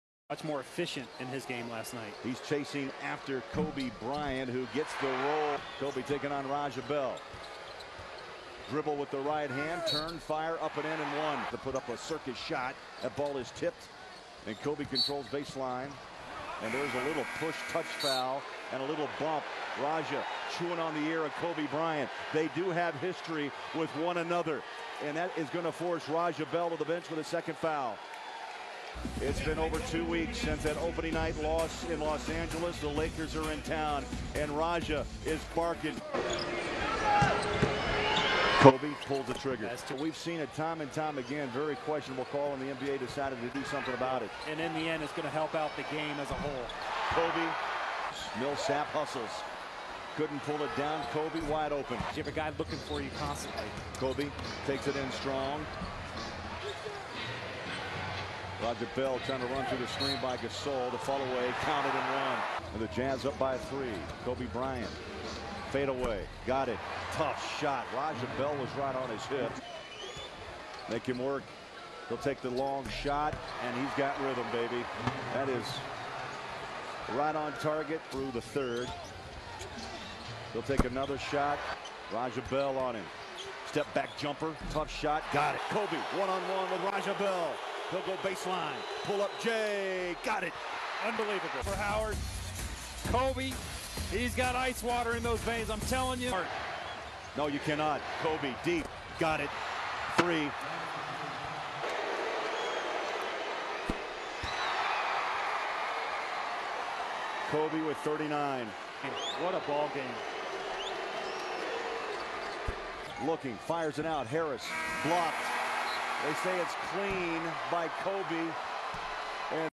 Kobe Bryant let Raja Bell knows he can't guard him
When Raja Bell Guarding Kobe Bryant
Kobe Bryant Highlights